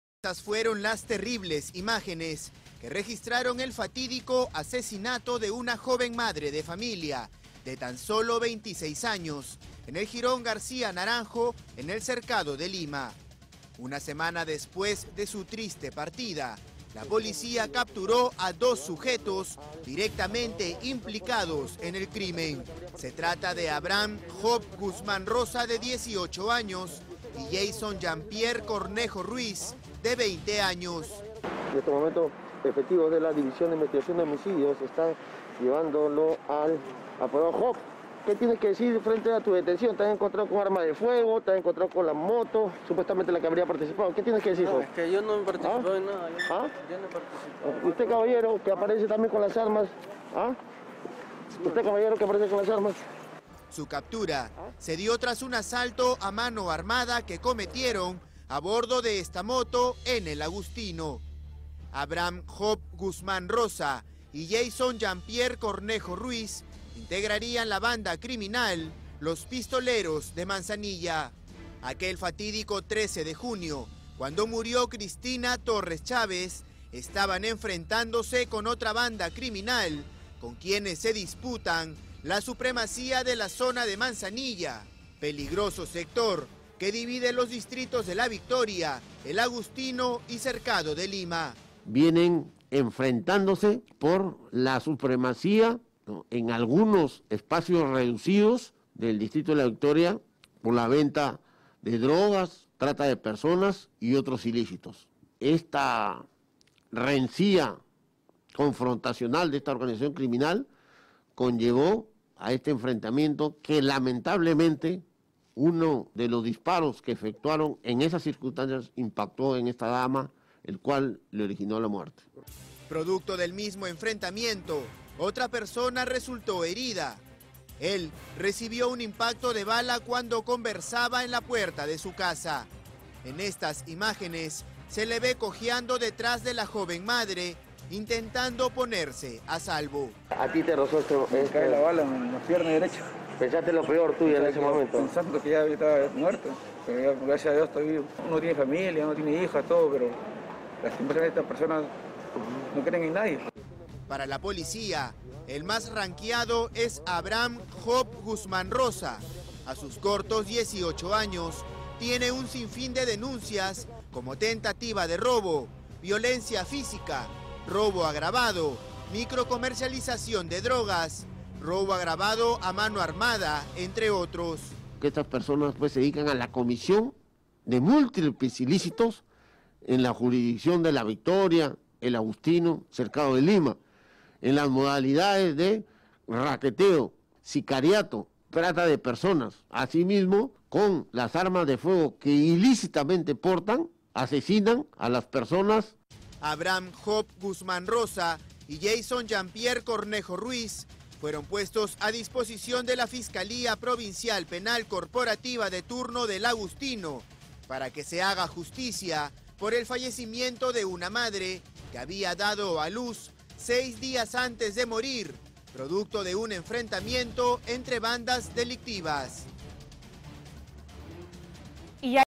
Asesinaron a joven madre mientras disputaban la venta de drogas en zonas de La Victoria
En el jirón García Naranjo, Cercado de Lima, una joven madre murió por una bala perdida mientras caminaba. Una semana después la Policía detuvo a dos implicados en el asesinato, Habrham Guzmán Rosa (18) y Jason Cornejo Ruiz (20), cuando realizaban un asalto a mano armada a bordo de una moto lineal.

Estos dos sujetos integrarían la banda criminal “Los pistoleros de Manzanilla”. El día en que murió la joven madre de 26 años, estos sujetos y su banda estaban enfrentándose con otra banda criminal para disputar la supremacía de la avenida de Manzanilla. “Vienen enfrentándose por la supremacía en algunos espacios reducidos del distrito de La Victoria por la venta de drogas, trata de personas y otros ilícitos”, señaló el coronel Víctor Revoredo, jefe de homicidios de la Dirincri.

“(…) Lamentablemente, uno de los disparos impactó en la dama, lo que le originó la muerte”, explicó el coronel Víctor Revoredo. Además, otra persona también fue impactada por las balas perdidas del enfrentamiento, afortunadamente, solo le rozó la pierna derecha. Los implicados fueron puestos a disposición de la Fiscalía Provincial Penal corporativa de turno de El Agustino.